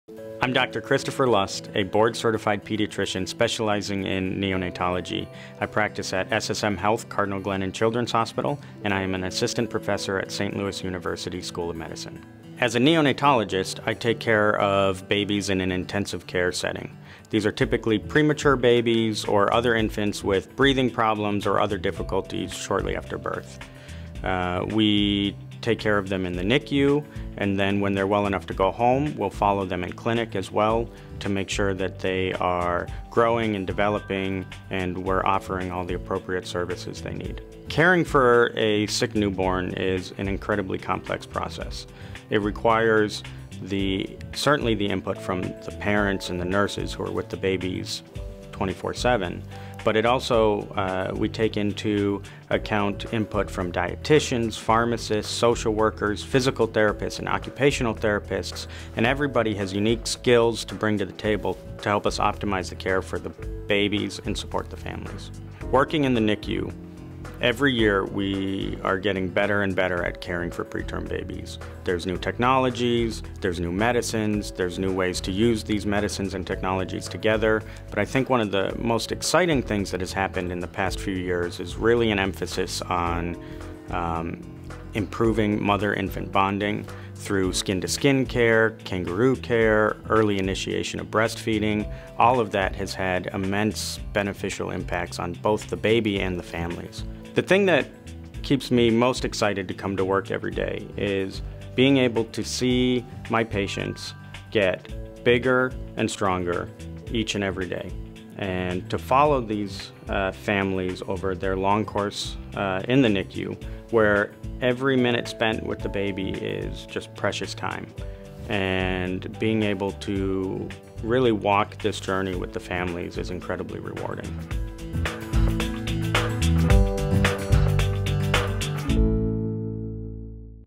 Christopher Lust, MD | SSM Health Cardinal Glennon Children’s Hospital
Dr. Lust is a board-certified neonatologist and takes care of babies in the NICU and works closely with nurses and other support staff to help babies grow and develop.